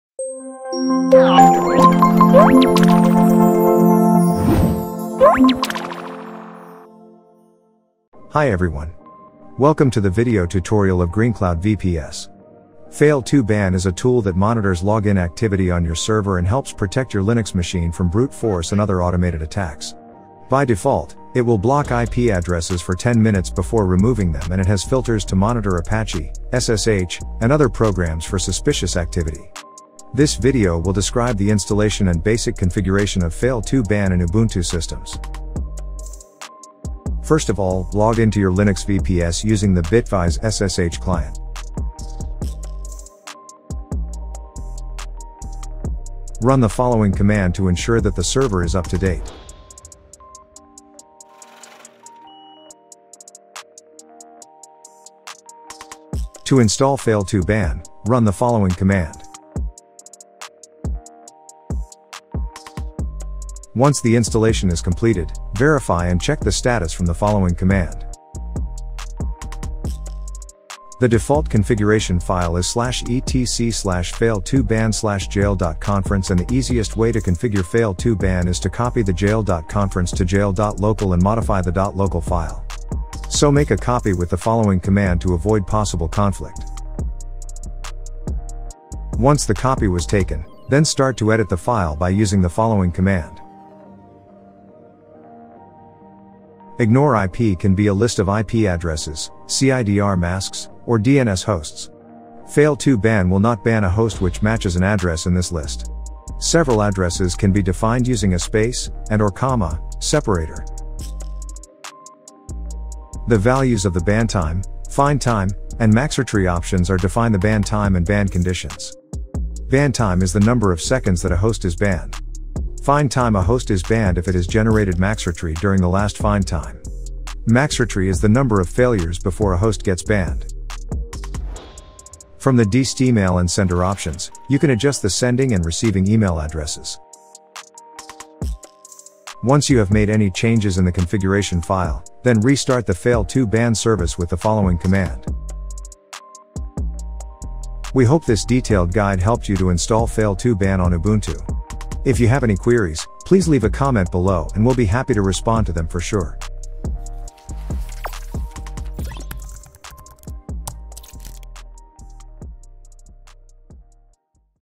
How to install and configure Fail2ban for Ubuntu | VPS Tutorial
Fail2ban is a tool that monitors login activity on your server and helps protect your Linux machine from brute-force and other automated attacks. By default, it will block IP addresses for 10 minutes before removing them and it has filters to monitor Apache, SSH, and other programs for suspicious activity.